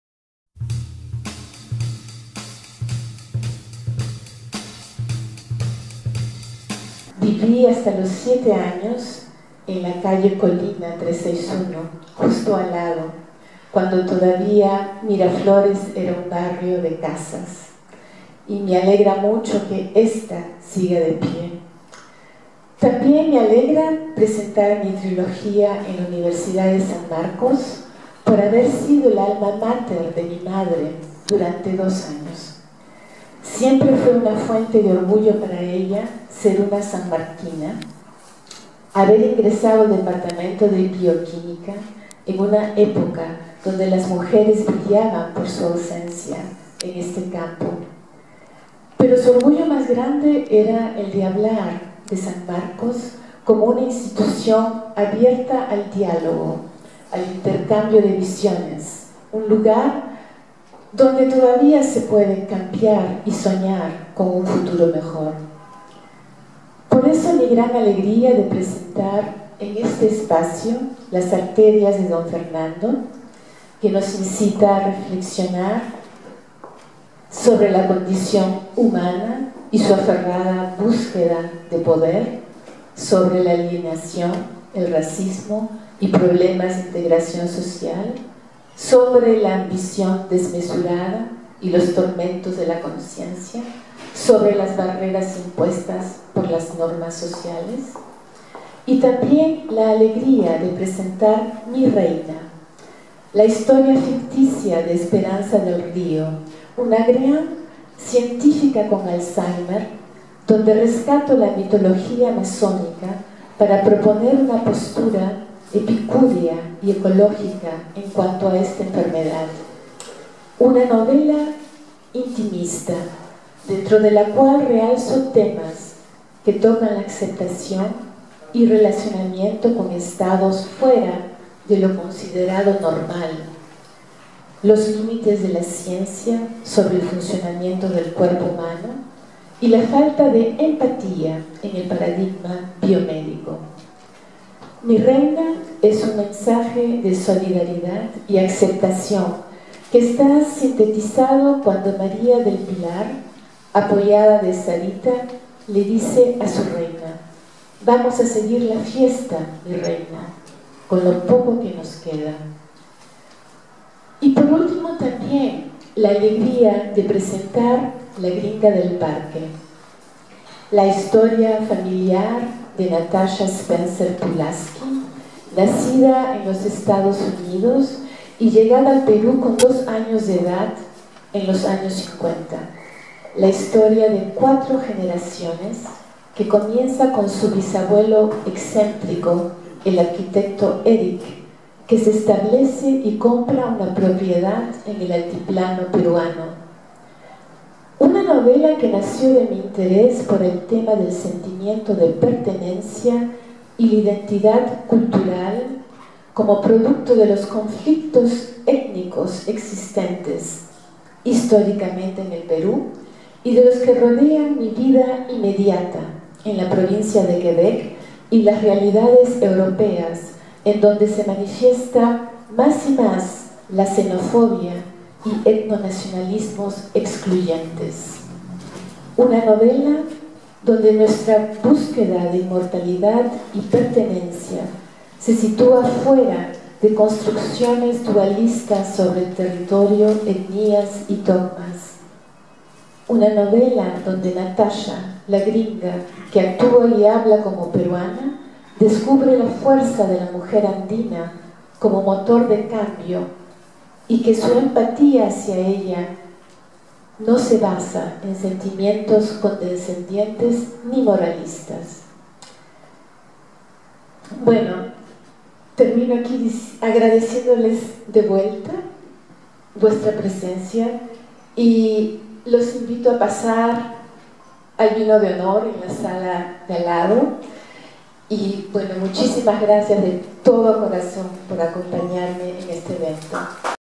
Gloria Macher en el Instituto Porras Barrenechea de la Universidad Nacional de San Marcos, Perú
Extracto de la presentación de la trilogia narrativa de Gloria Macher compuesta por  Las arterias de don Fernando, Mi reina y La gringa del parque en el Instituto Raúl Porras Barrenechea de la Universidad Nacional de San Marcos, Lima, Perú

El jueves 4 de agosto de 2016 a las 7 pm.
Estas tres obras, influidas por la percepción de la escritora sobre la sociedad peruana, indagan las realidades sociales de la costa, sierra y selva del Perú, país donde la escritora vivió su infancia. Gloria Macher explora temas de contenido social con diversos matices, revindicando valores anclados y poniendo en evidencia las tensiones y desacuerdos entre los miembros de una misma sociedad.